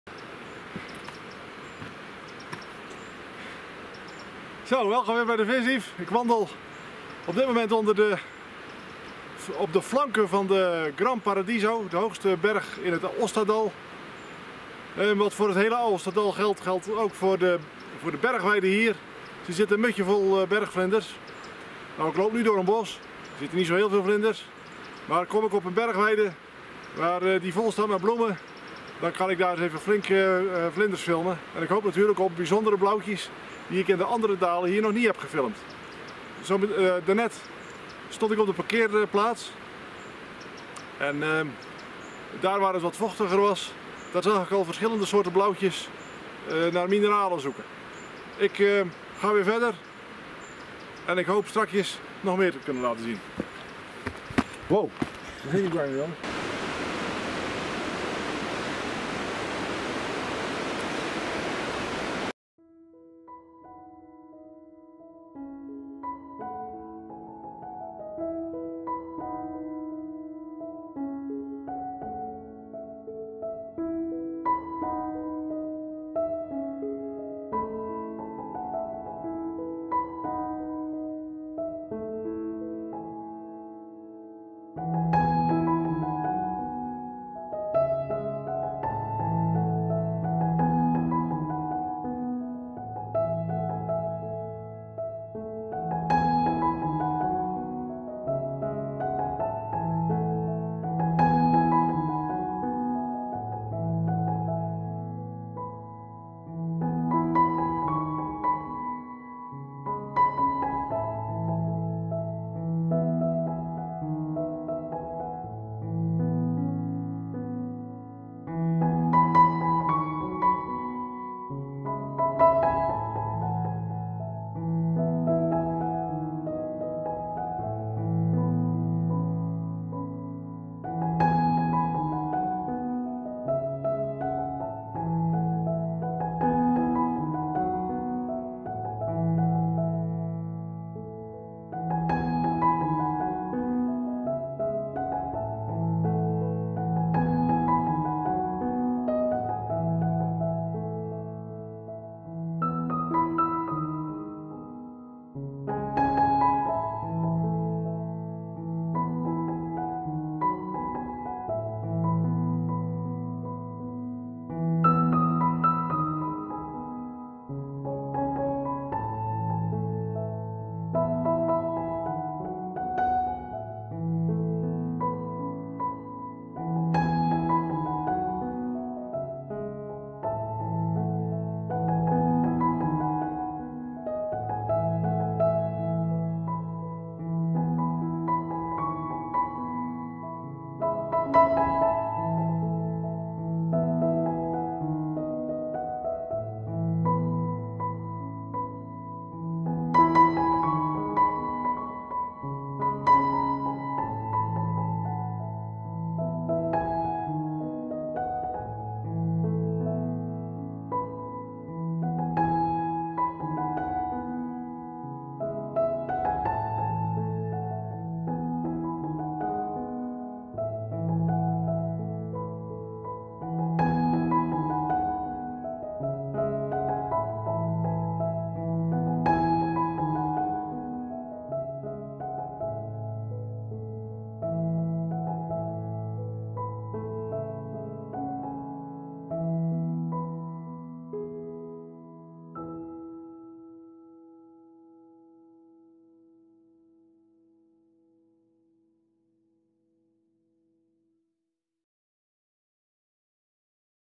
Vlog N°30 Vlinders kijken bij Valsavarenche
Achter het dorp Valsavaranche is het heerlijk wandelen en vlinders kijken. En altijd kijk je uit op de Gran Paradiso, de hoogste berg van de regio. Lees mijn verhaaltje bij dit filmpje op

Correctie
De zwarte apollovlinder blijkt bij nadere inspectie geen zwarte apollovlinder, maar een 'gewone' apollovlinder. Te zien aan de zwarte vlek midden op de voorvleugel en aan de vage rode vlekken die door de bovenvleugel doorschemeren. Deze details had ik over het hoofd gezien.

En het alpensaffierblauwtje is mogelijk een heideblauwtje. Het is te ver afgevlogen om hem met zekerheid te kunnen determineren. Ik twijfel sterk en neig ernaar om hem als een heideblauwtje te zien. Reageer gerust als je een verlossend woord kunt spreken.